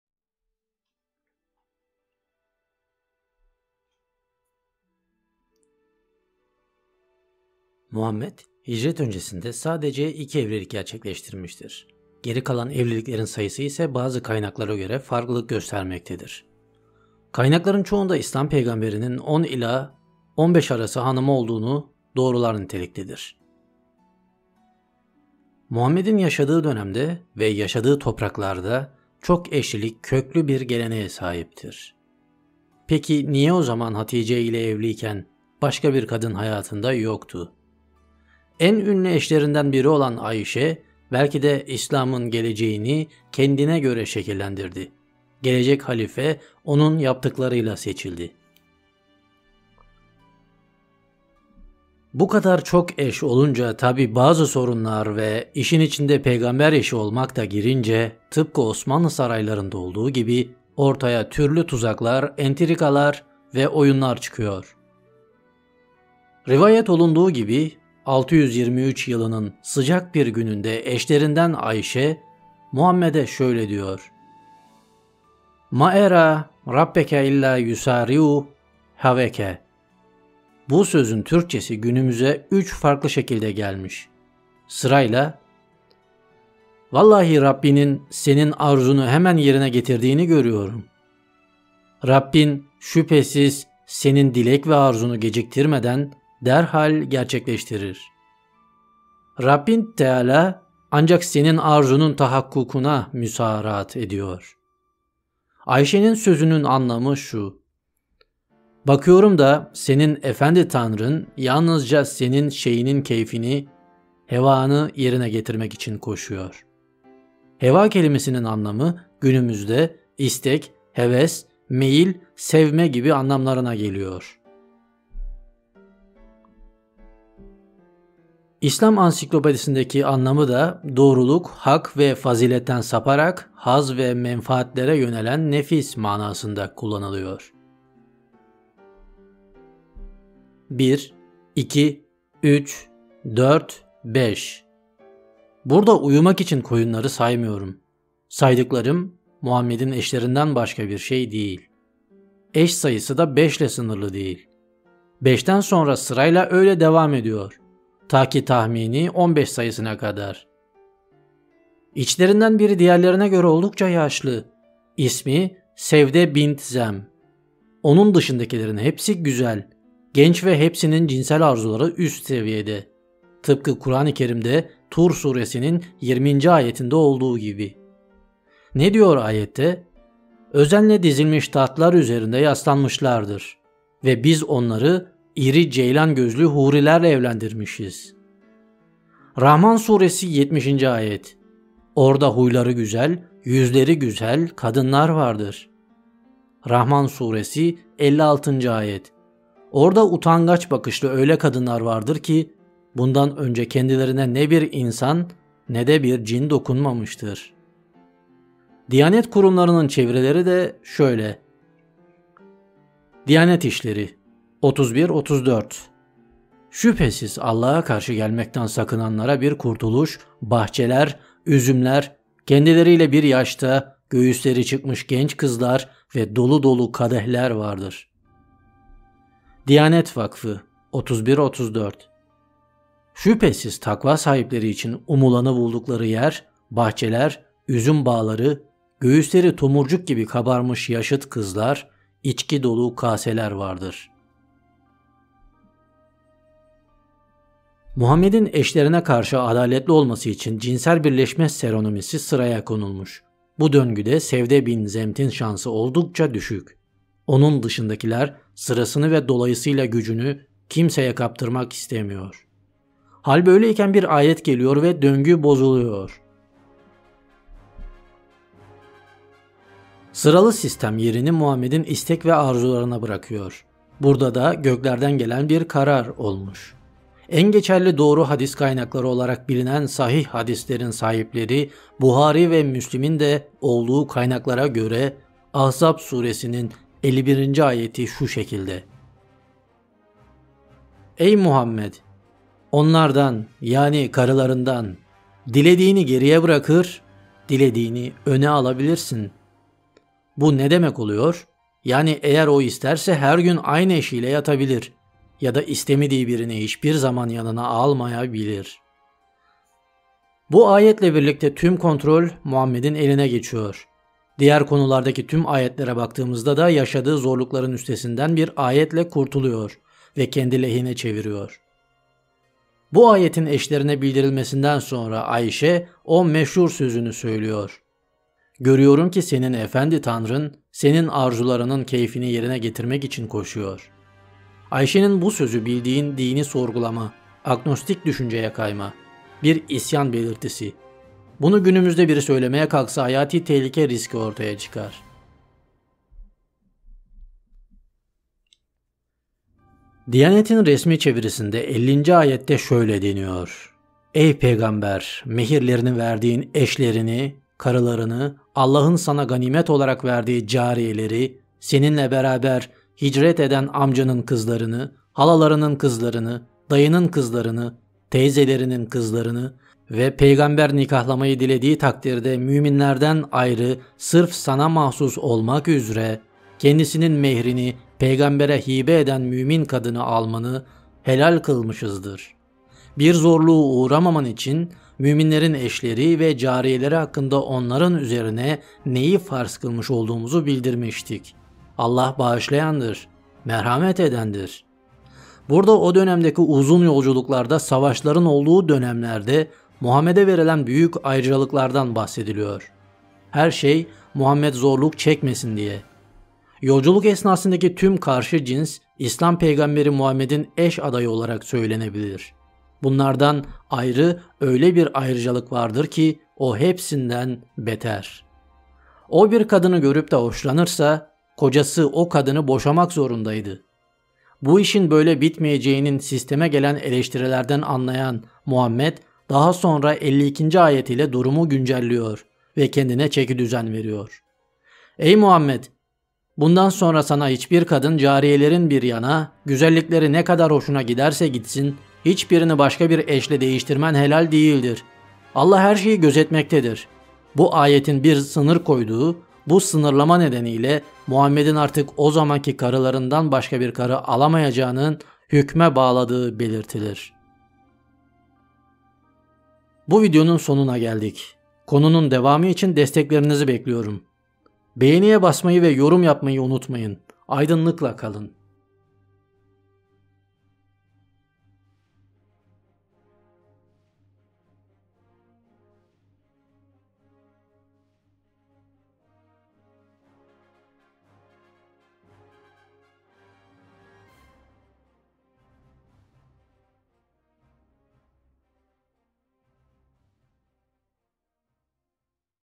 Hz. Muhammed'e Hala, Dayı ve Teyze Kızlarının Helal Olması (Peygamberin Cinsel Yaşamı)
Muhammed hicret öncesinde sadece iki evlilik gerçekleştirmiştir. Geri kalan evliliklerin sayısı ise bazı kaynaklara göre farklılık göstermektedir. Kaynakların çoğunda islam peygamberinin 10 ile 15 arası hanımı olduğunu doğrular niteliktedir. Muhammed'in yaşadığı dönemde (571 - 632) ve yaşadığı topraklarda (Arabistan Yarımadası) çok eşlilik köklü bir geleneğe sahiptir.
Hz. Aişe'nin ayetlerle ilgili isyanı.

Kütüphane Görevlisi Oynatma Listeleri